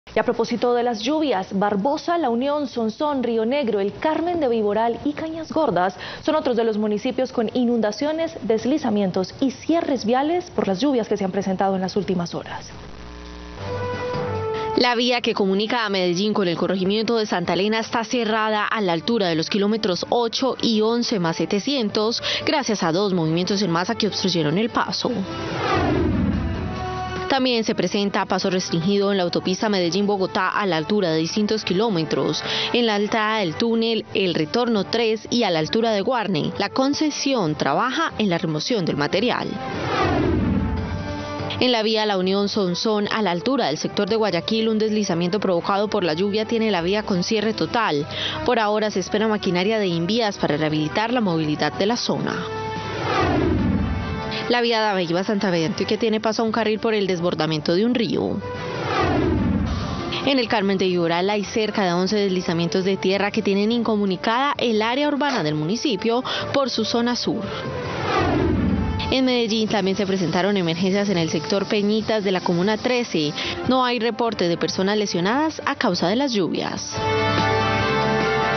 Conozca las vías cerradas, tras las fuertes lluvias en Antioquia
Las lluvias de este fin de semana dejan varias afectaciones en zonas del departamento.